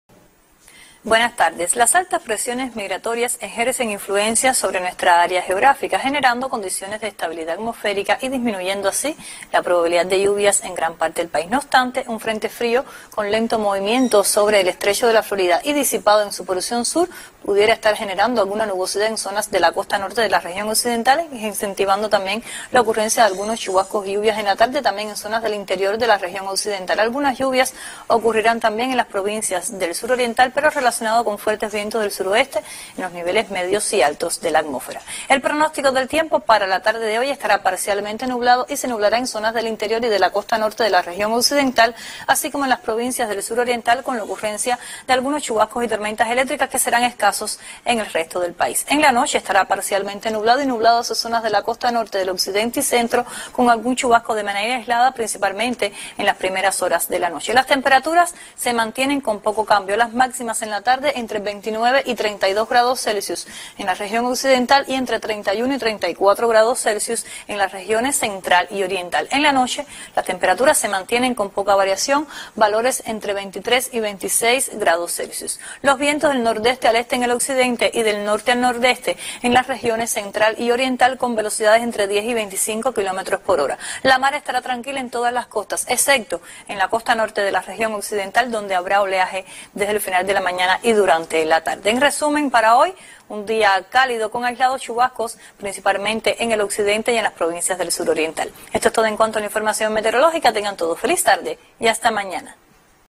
Pronóstico del tiempo en Cuba: algunas lluvias en occidente y el sur oriental
El Instituto de Meteorología (INSMET) de Cuba pronostica que desde el final de la mañana estará parcialmente nublado en gran parte del país y se nublará en la tarde en algunas localidades del interior de occidente y en el sur oriental, con algunos chubascos y tormentas eléctricas, que serán más aislados en el resto del país. En la tarde se alcanzarán temperaturas máximas entre los 29 y 32 grados Celsius en occidente y centro, superiores en el sur oriental. En la noche las temperaturas estarán entre los 23 y 26 grados Celsius. Los vientos serán variables débiles, desde el final de la mañana soplarán del nordeste al este en occidente y del norte al nordeste en el resto, con velocidades entre 10 y 25 kilómetros por hora, que serán superiores en la costa norte occidental. La mar estará tranquila en todos los litorales, excepto en el norte occidental donde habrá oleaje desde la tarde.